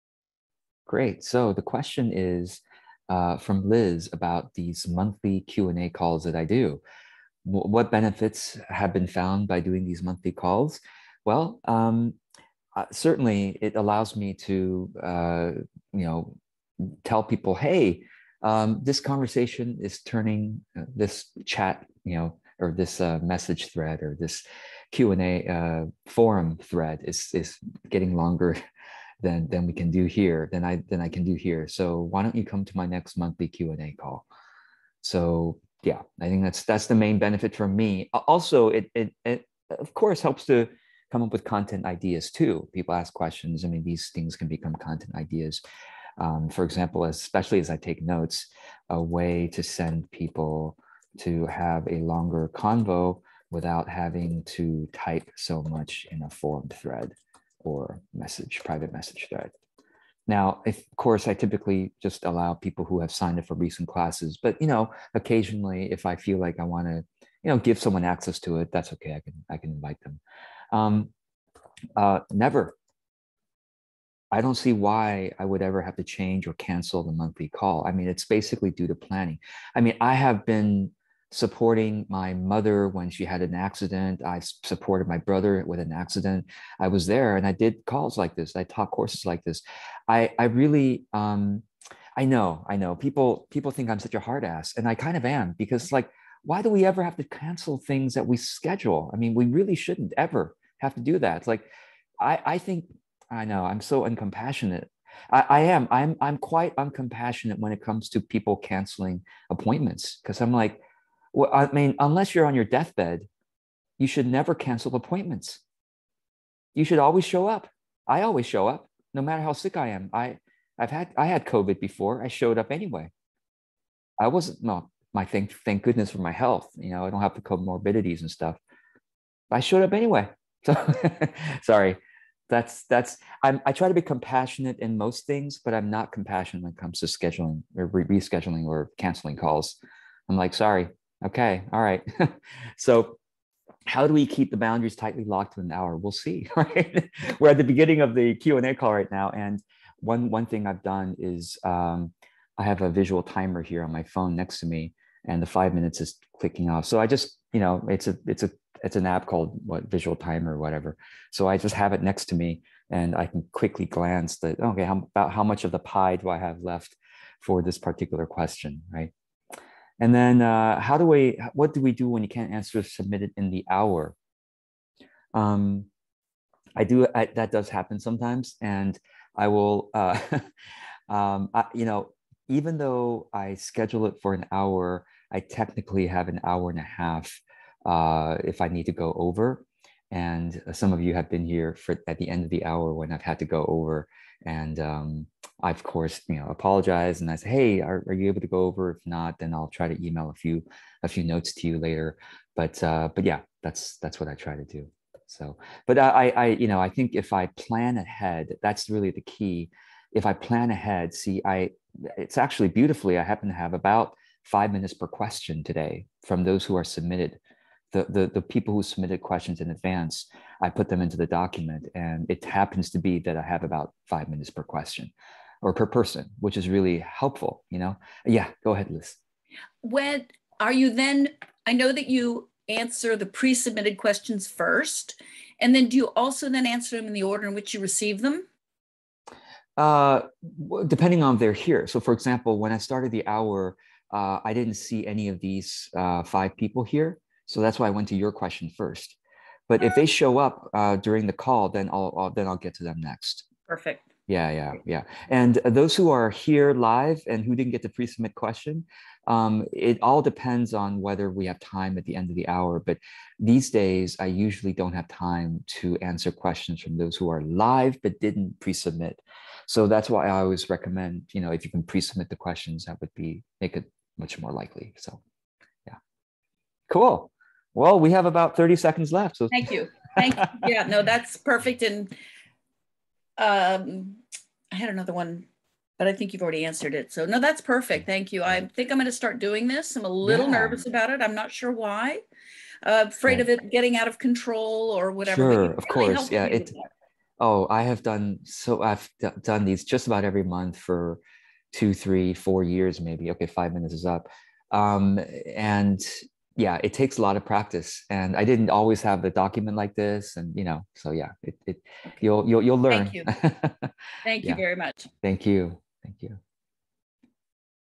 About my Monthly Bonus Q&A calls -- why I do them (and why I always show up)
This is a segment from my Monthly Live Q&A webinar. You get access to 2 months of these when you sign up for any of my courses